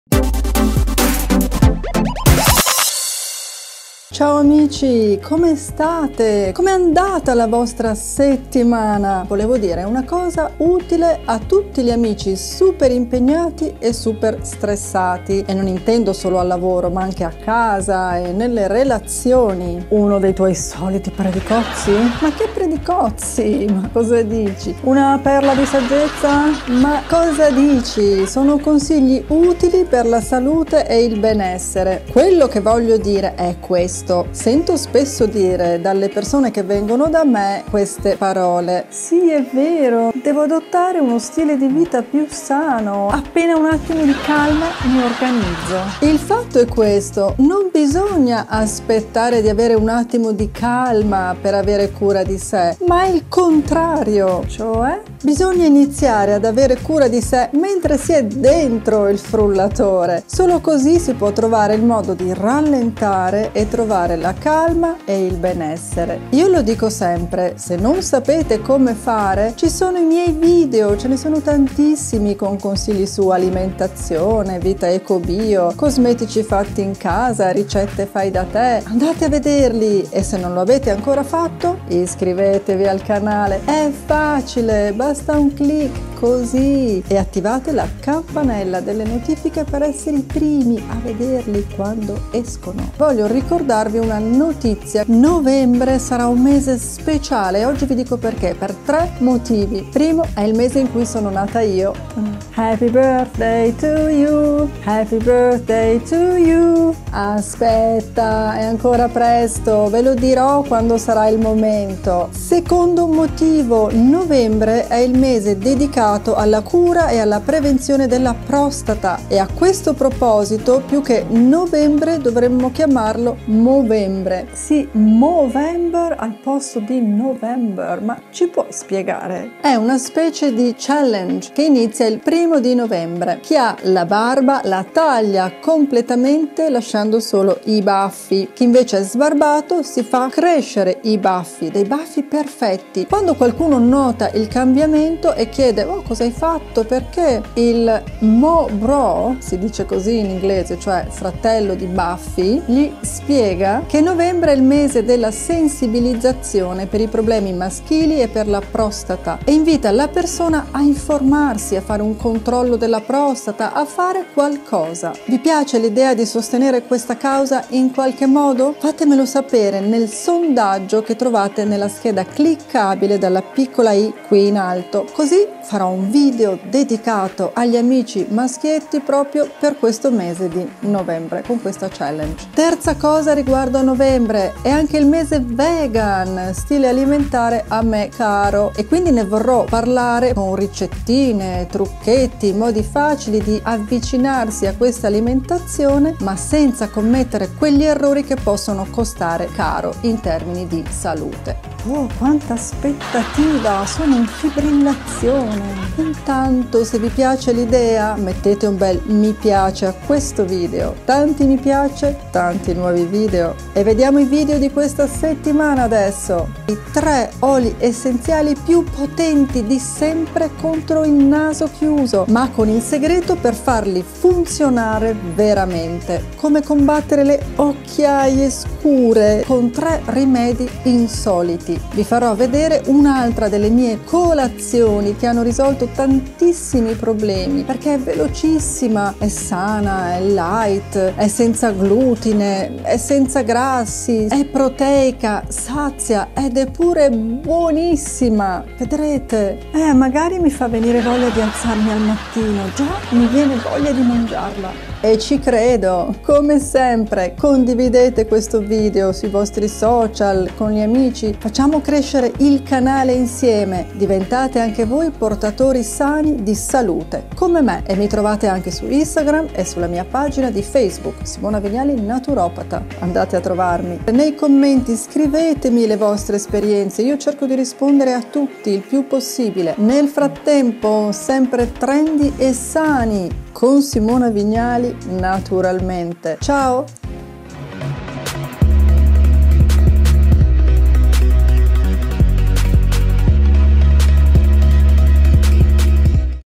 TRUCCHI ANTI STRESS e ANTI OCCHIAIE, NOVEMBRE MESE della PROSTATA e MOVEMBER CHALLENGE
Ciao amici! Per questa settimana, ecco i miei video che vi consiglio di guardare. Nel video trovate anche 1 consiglio anti stress e tutte le info sulla movember challenge che inizia a Novembre per la prevenzione della prostata.

SEMPRE in FORMA con la NATURA
con il mio MANUALE di NATUROPATIA

Nota bene

Simona Vignali